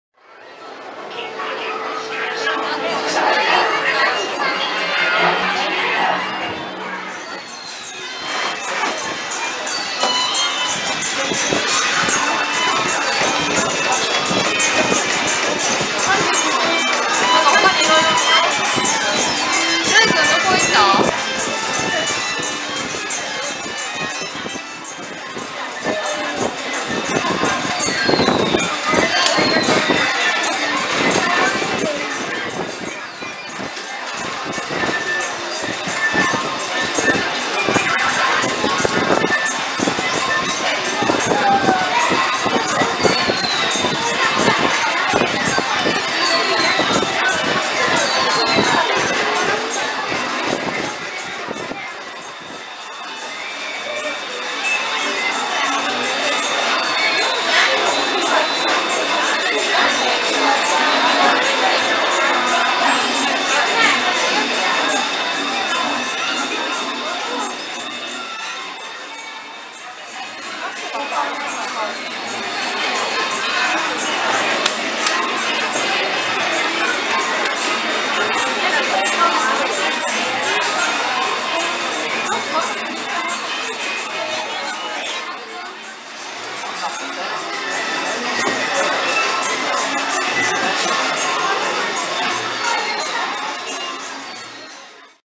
【ちょっとだけ行った気分★ギャグファクトリー】東京ディズニーランド Tokyo Disneyland Toon Town GAG
東京ディズニーランドの店舗施設はどれも凝ったものが多いですが、その中でもトゥーンタウンにある店舗は得に仕掛けも多く、見ていても飽きません。

今回は、トゥーンタウンギャグファクトリー(ファイブアンドダイム)の店舗の様子です。